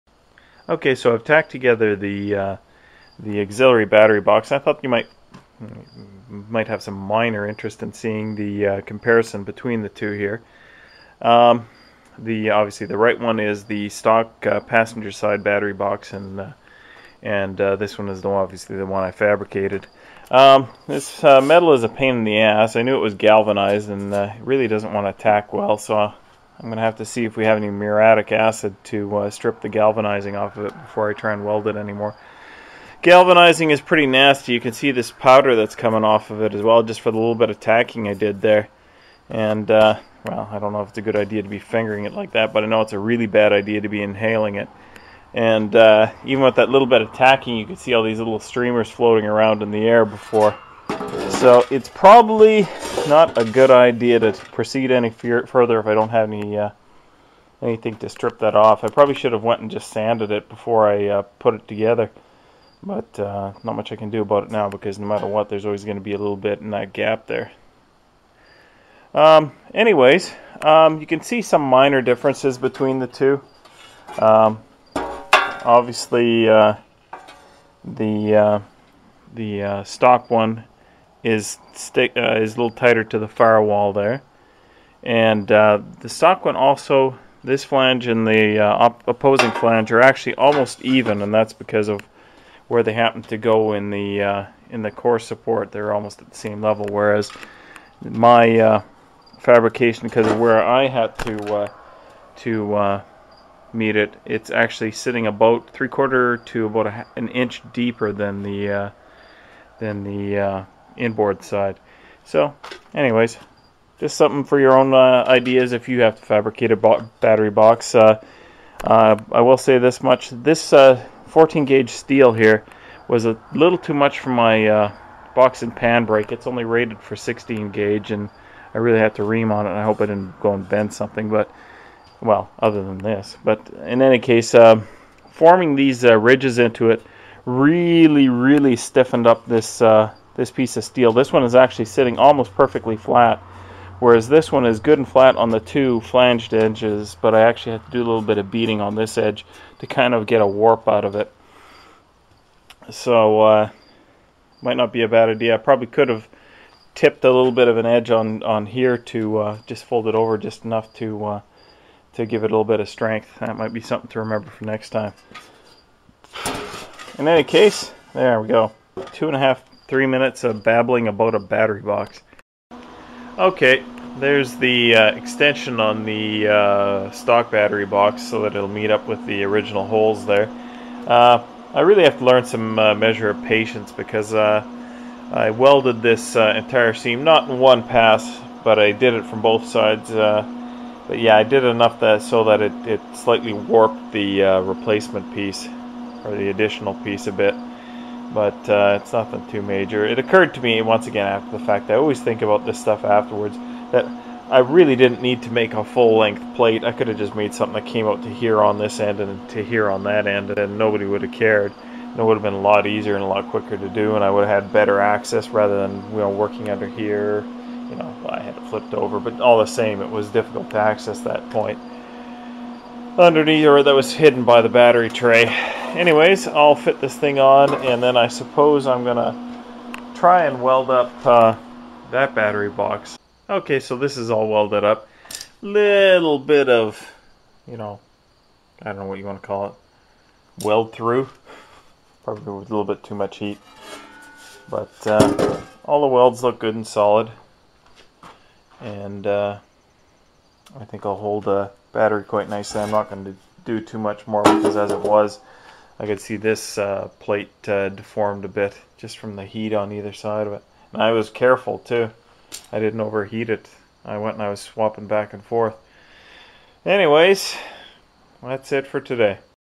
1972 K25 - Enough of the Auxillary Battery Tray
Ok I'm done the auxillary battery box, I won't mention it again.

Don't forget to click the "Like" button below it helps promote my videos.